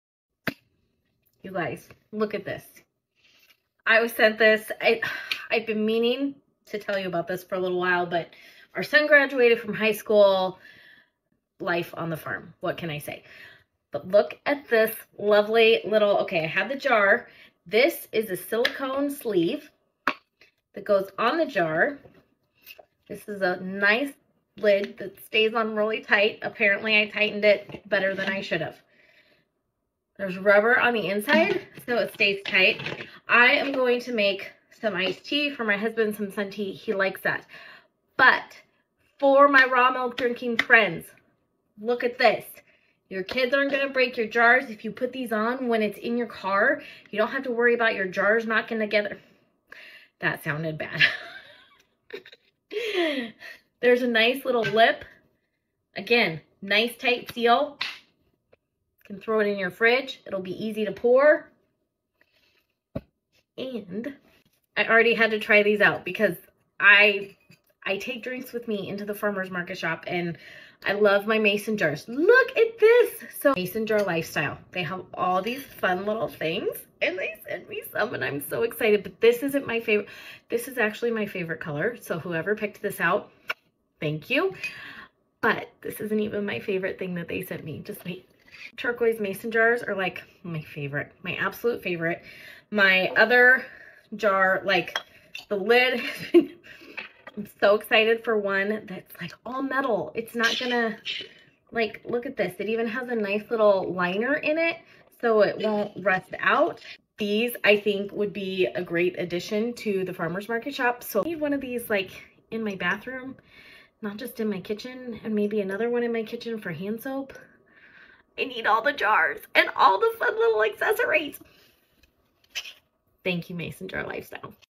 Milk Jar Hack: Keep your jugs from knocking together 😆 and so many mason jar accessories for the hom
Milk Jar Hack: Keep your jugs from knocking together 😆 and so many mason jar accessories for the homestead house and kitchen. Thank you Jar Lifestyle for the goodies!!!